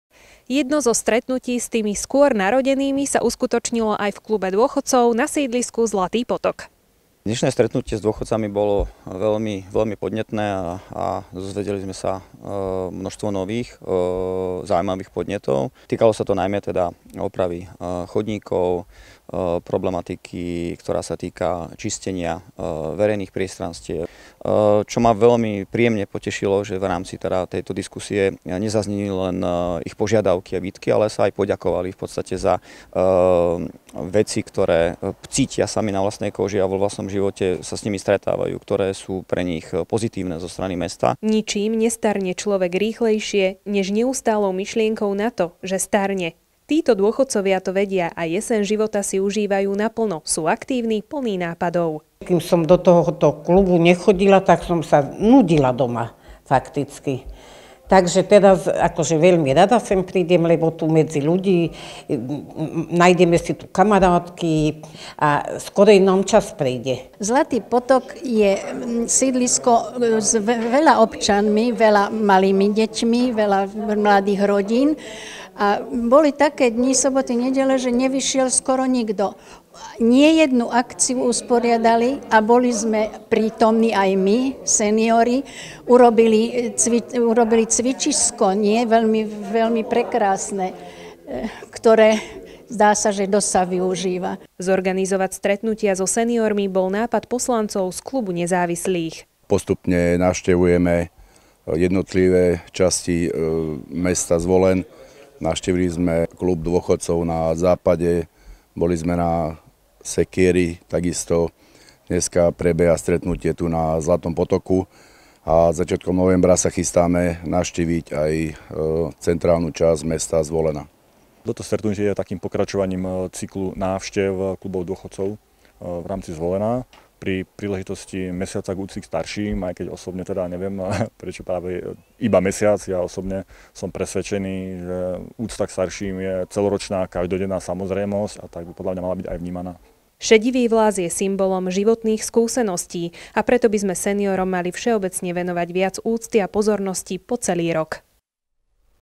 STRETNUTIE S DÔCHODCAMI (Zvolen)
Mesiac úcty k starším je za nami. V rámci októbra venovali väčšiu pozornosť tým skôr narodeným aj vo Zvolene.